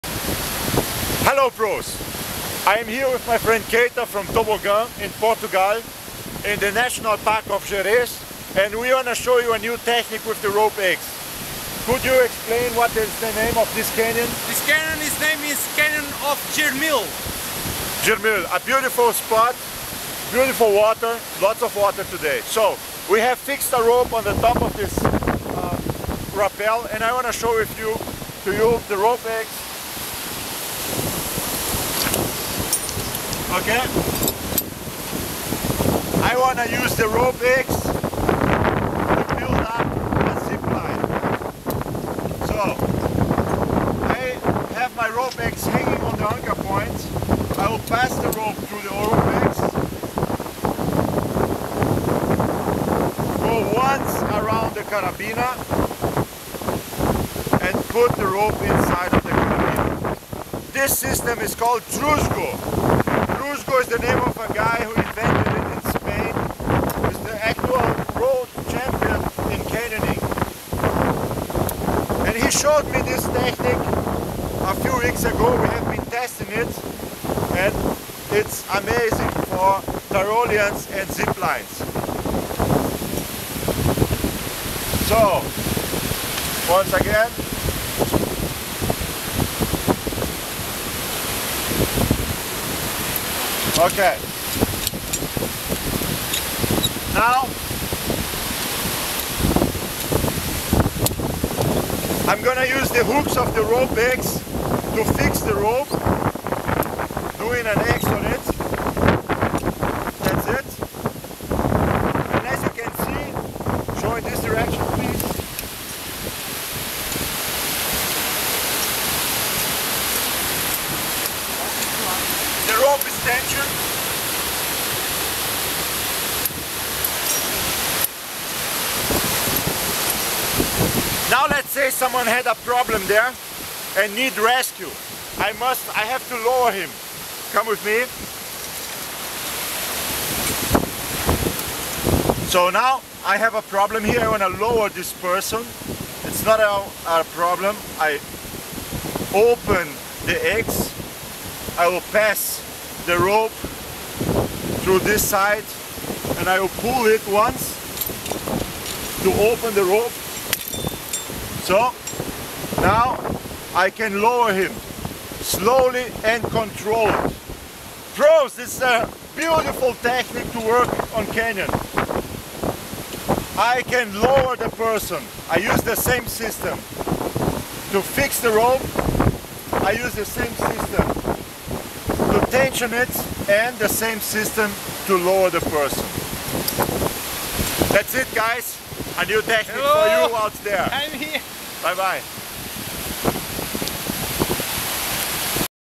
ZipLine Rope X
Rope X Technik, um Seilbahn mit lösbare und ablassbare rücklaufsperre aufzubauen. Ursprungstechnik adaptiert von Jorge Trasgu aus Spanien.